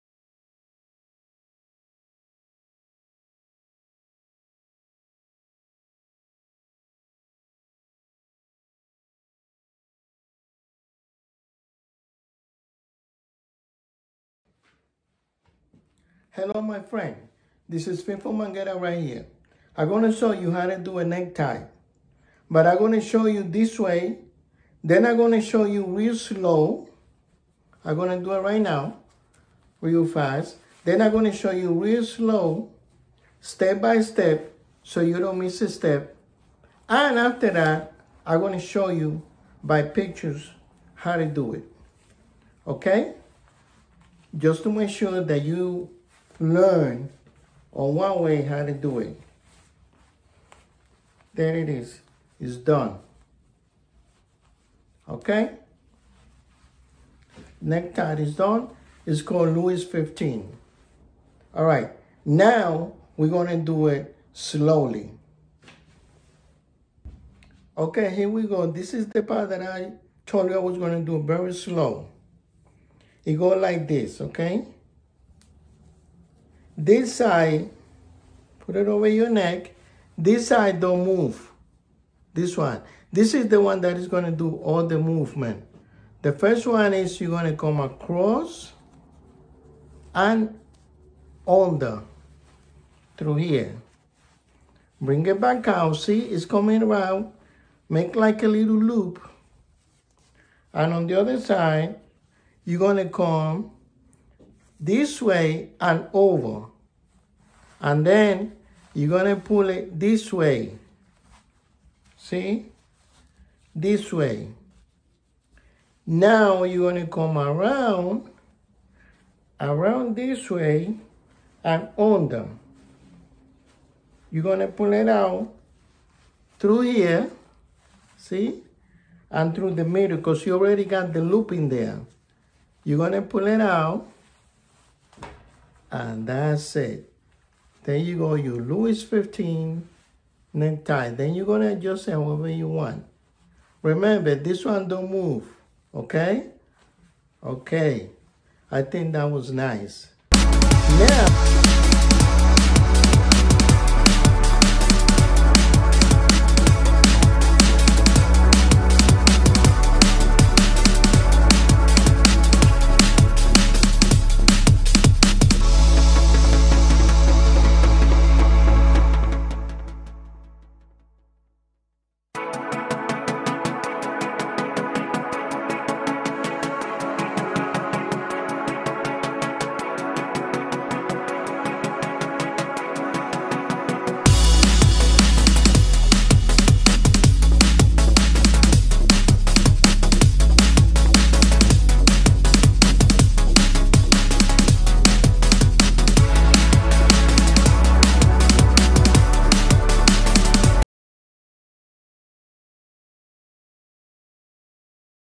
NECK TIE 3 EASY STEPS
Two videos one regular , one slow, and then we are going to show you the steps in pictures or photos.
If you have ant questions let me know on the comments session or by email, I will respond as soon as possible. Thanks.